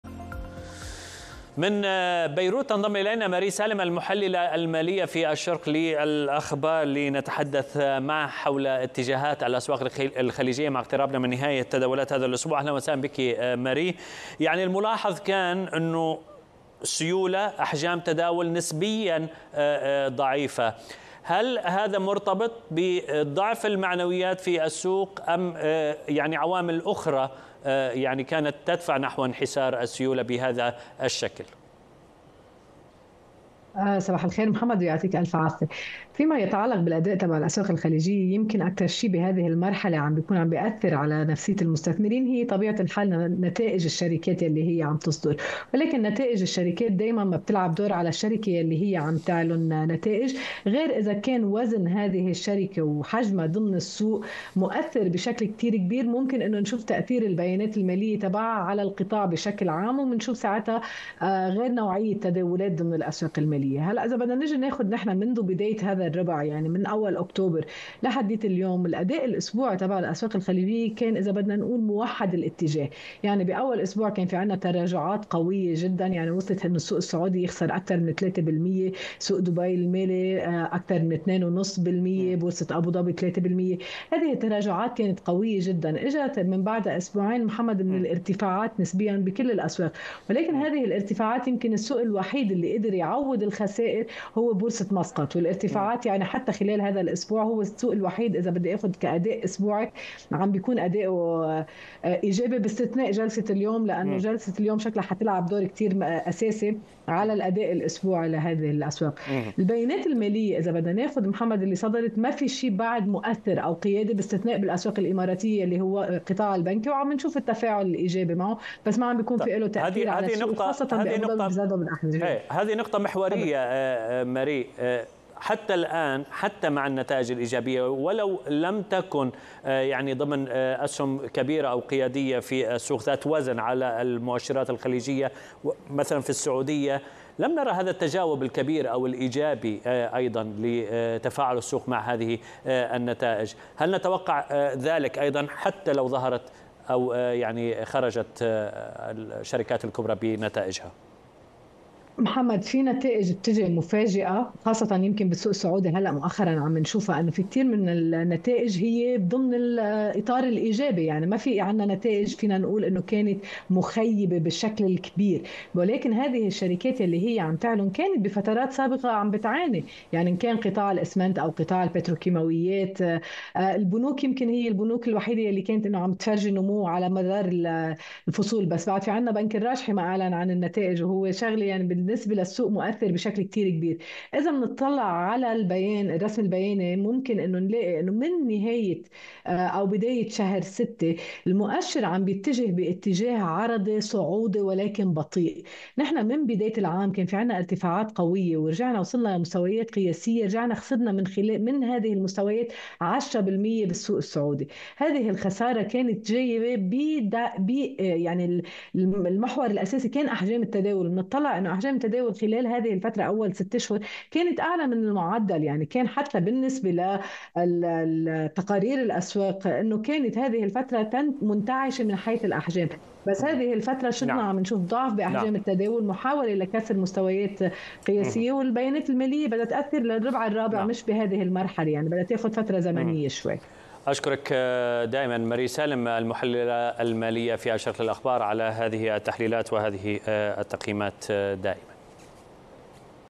اقتصاد الشرق - مؤشرات الشرق - 24/10/2024
نظرة على الأداء الأسبوعي للأسواق الخليجية وتركيز على اتجاهان السوق السعودي وتقلباته